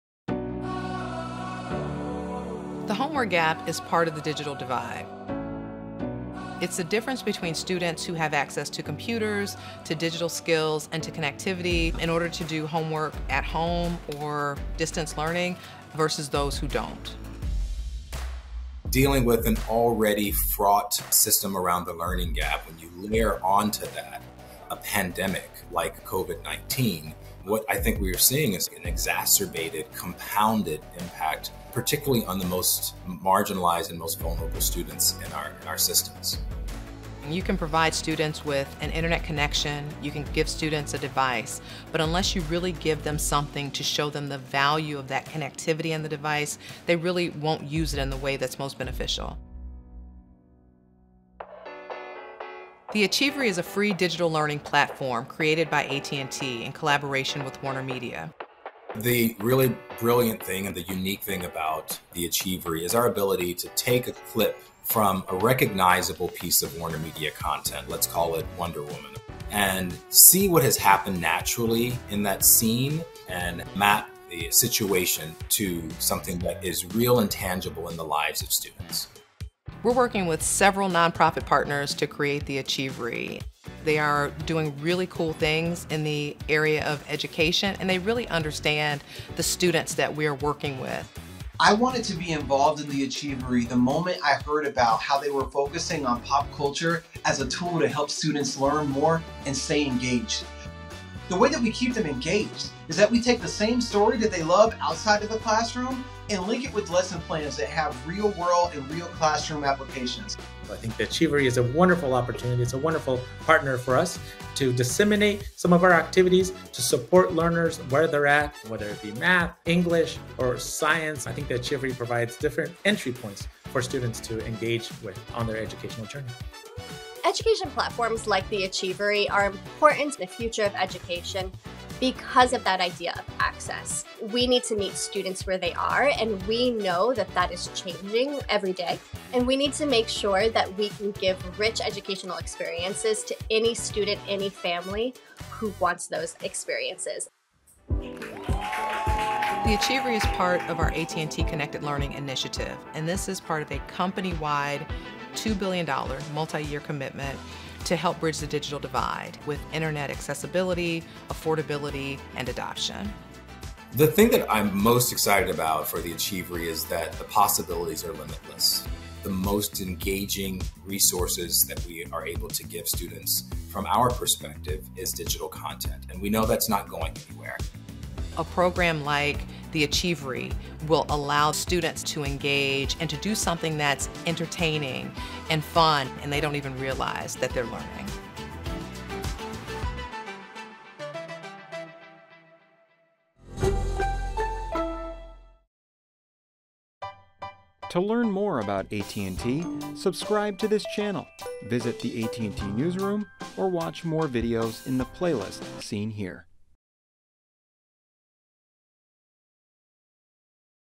Students K-12 Excel with The Achievery, Created by AT&T | AT&T Newsroom
The Achievery aligns with common core curriculum, social-emotional learning, English language arts and the STEM curriculum.

About AT&T:
Laugh, learn, discover. See your favorite products, services, customer stories, entertainment, and our innovations in action. Watch the new connected life unfold.